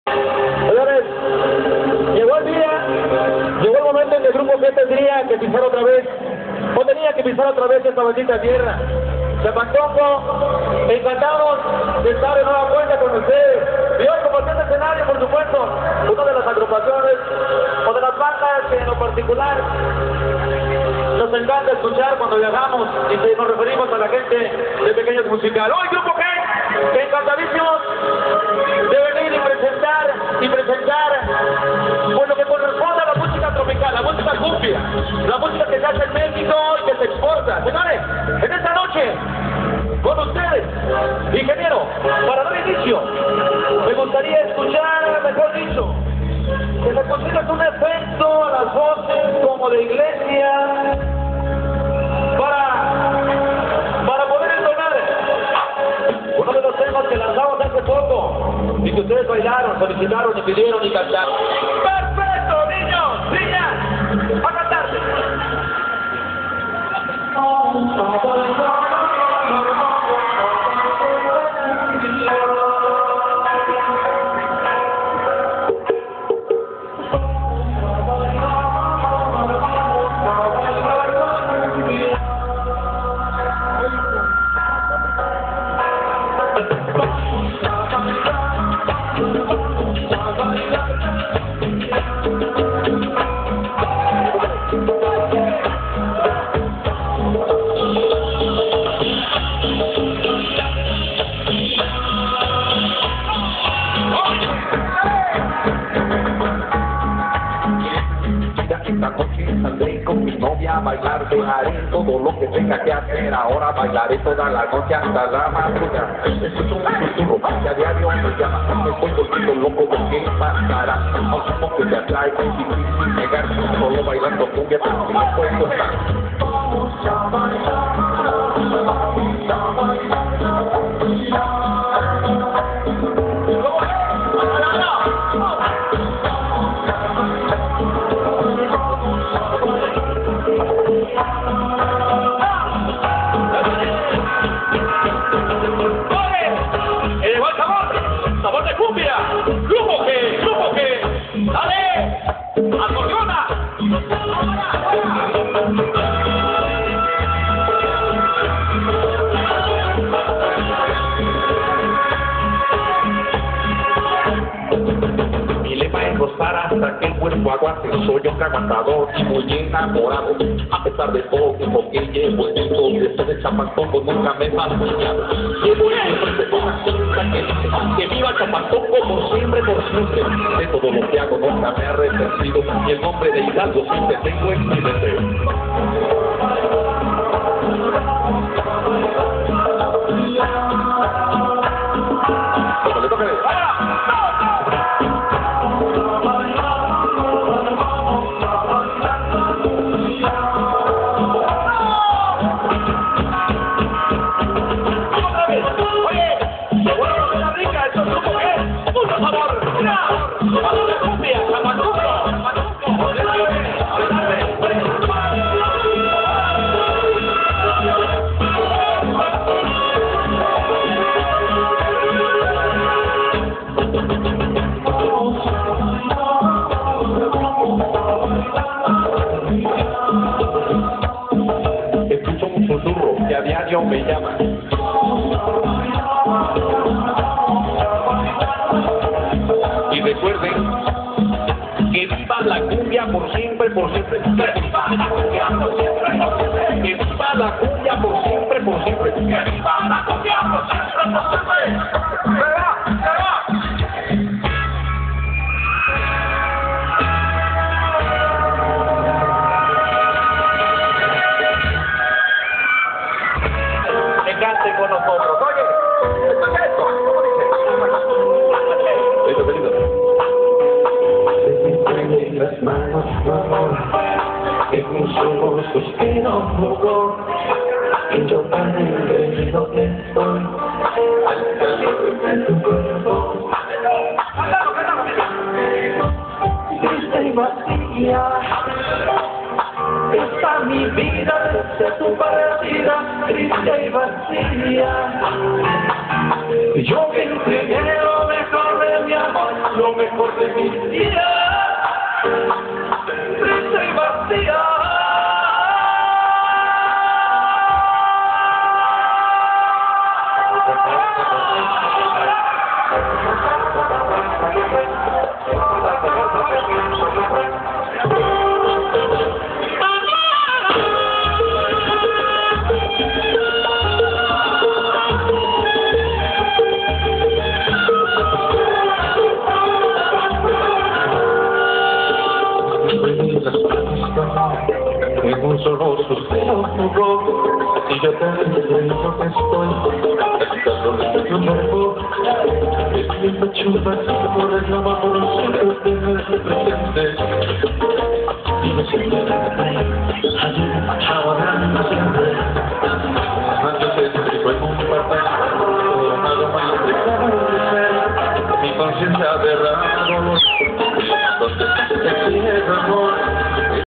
super grupo g en chapantongo
super grupo "g" en chapantongo hgo en el bailazo de feria alternando con la banda mas romantica de america PEQUEÑOS MUSICAL